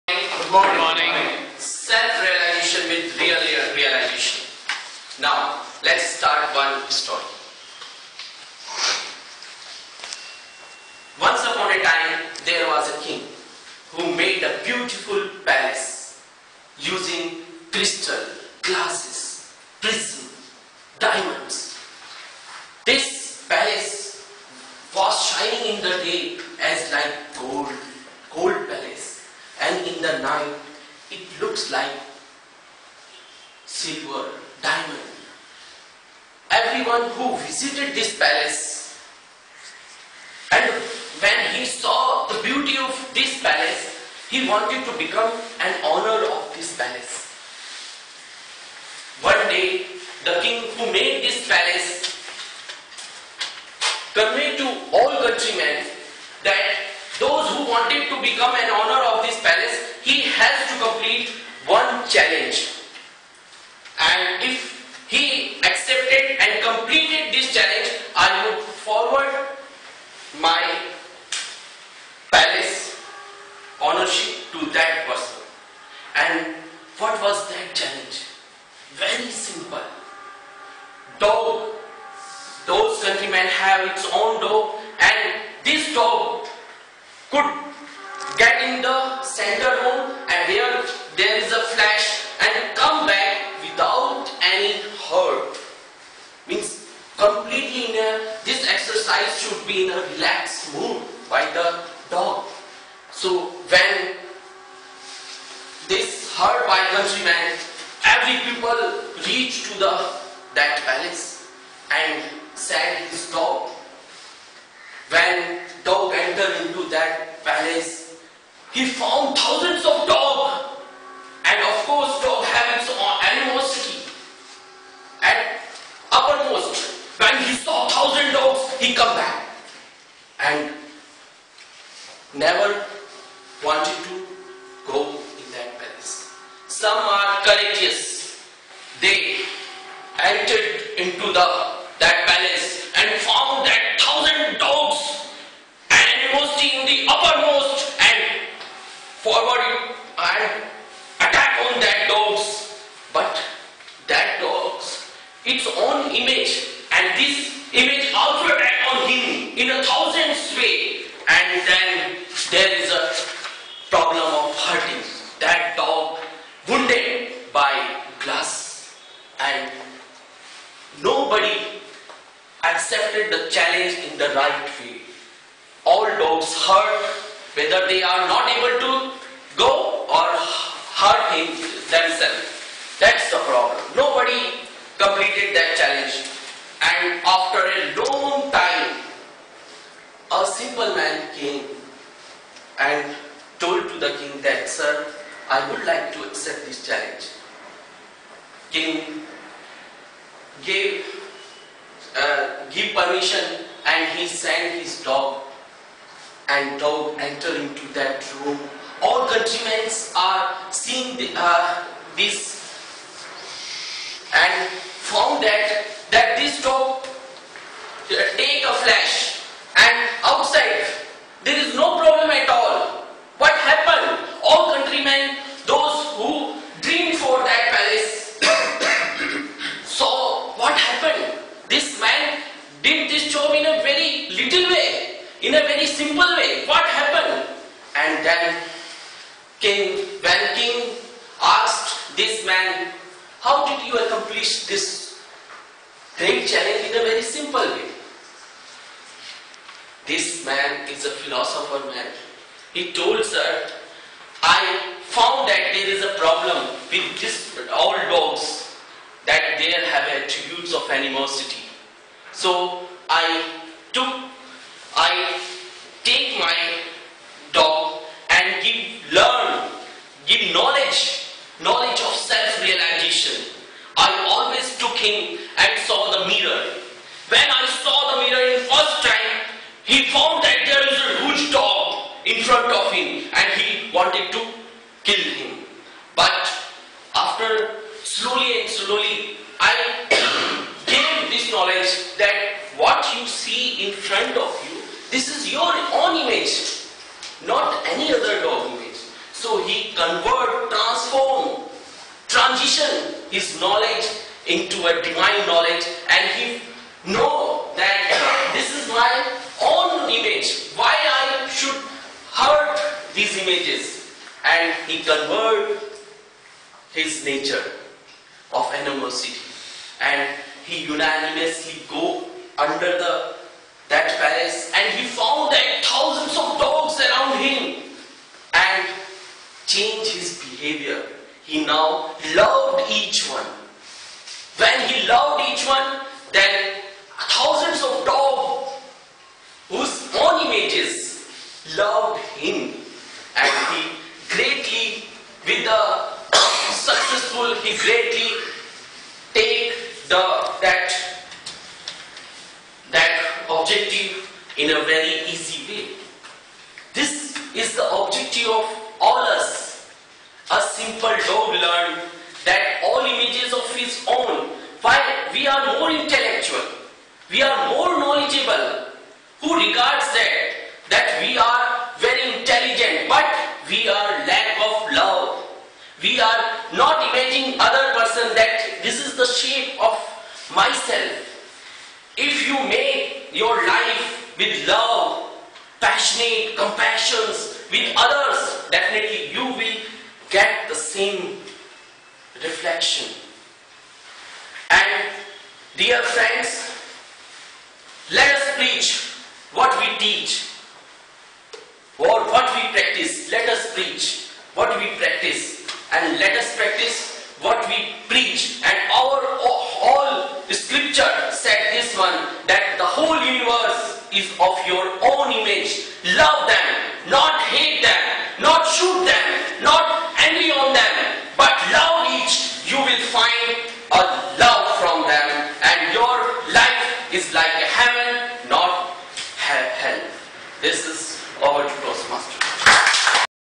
Toastmaster Rajesh Tyagi P9 " Self realization with Real realization"
Toastmaster club of Dwarka 156th Meeting . Theme "Christmas"